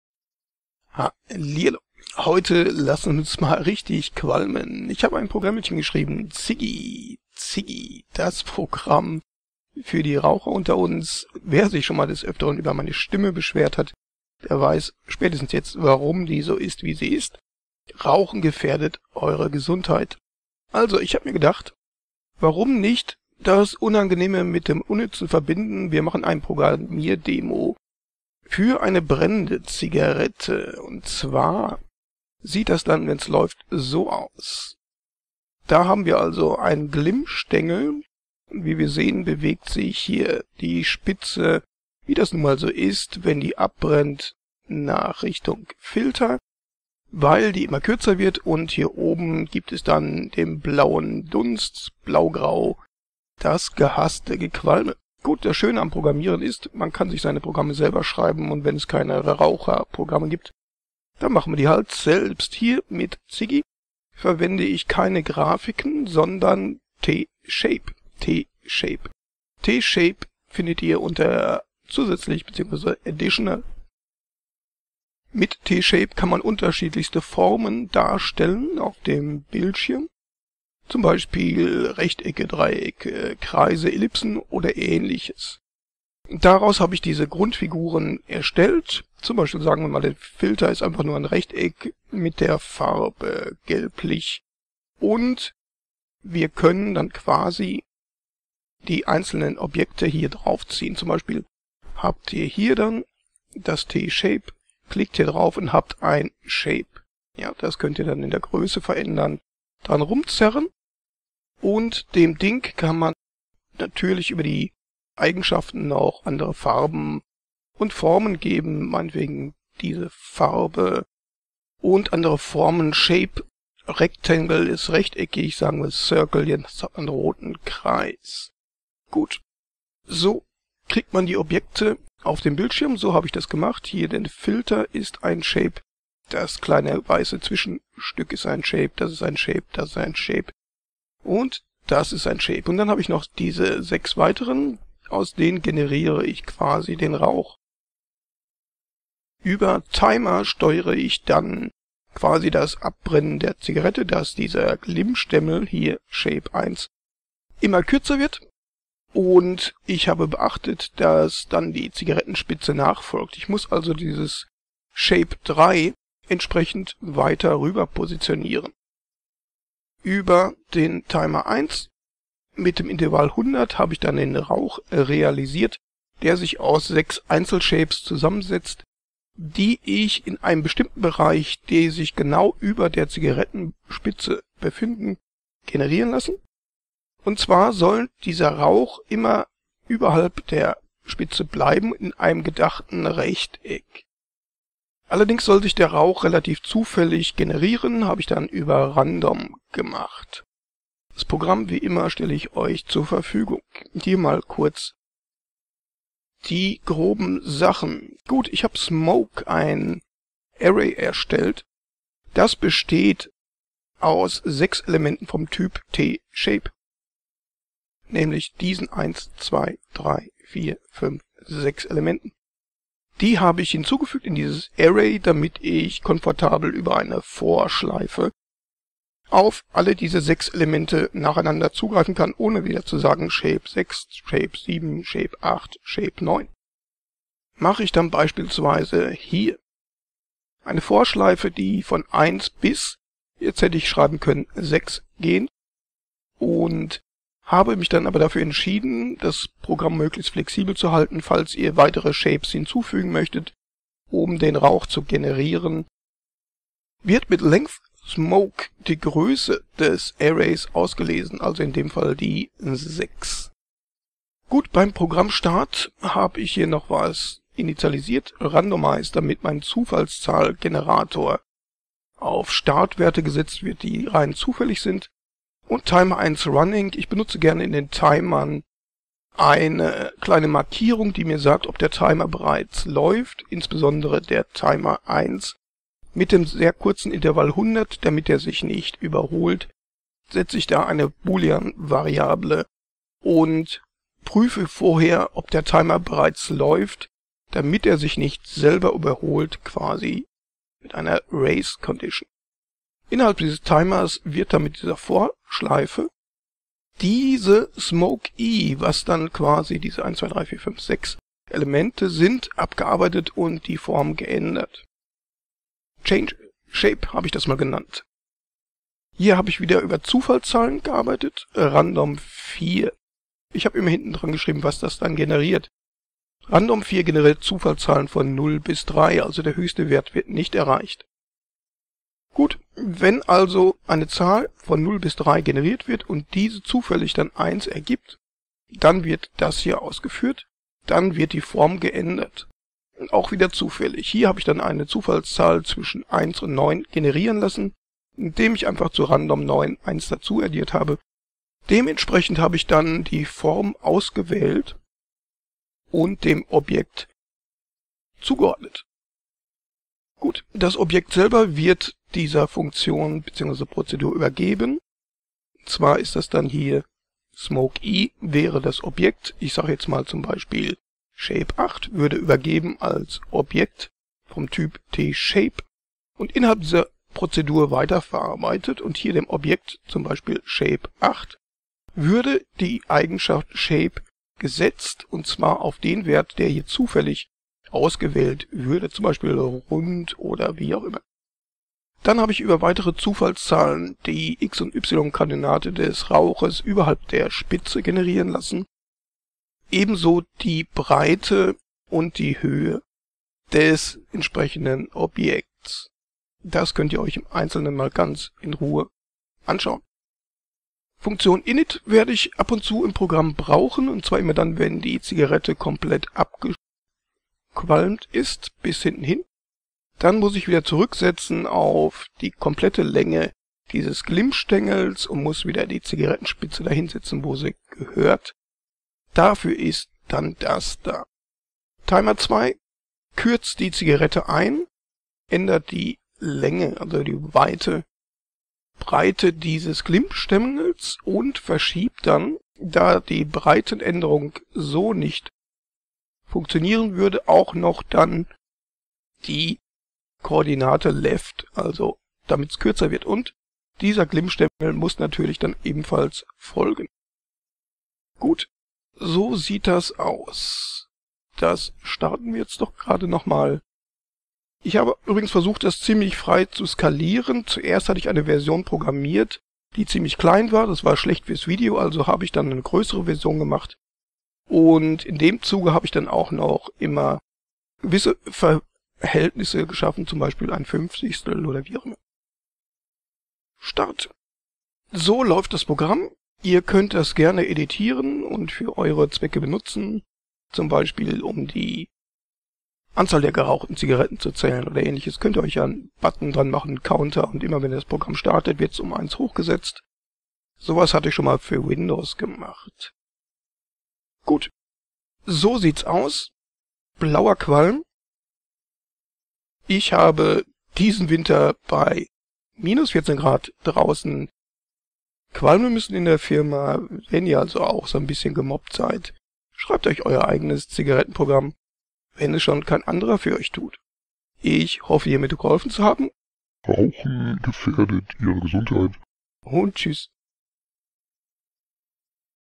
Lazarus[Delphi]:= array of TShape; // Wir lassen es qualmen ;-)
Meine Zigaretten-Rauch-Simulation unter der Programmiersprache "Lazarus" die "Delphi" sehr ähnlich ist.

Open Source Quelltext zum downloaden:
"Ziggi-Lazarus.zip"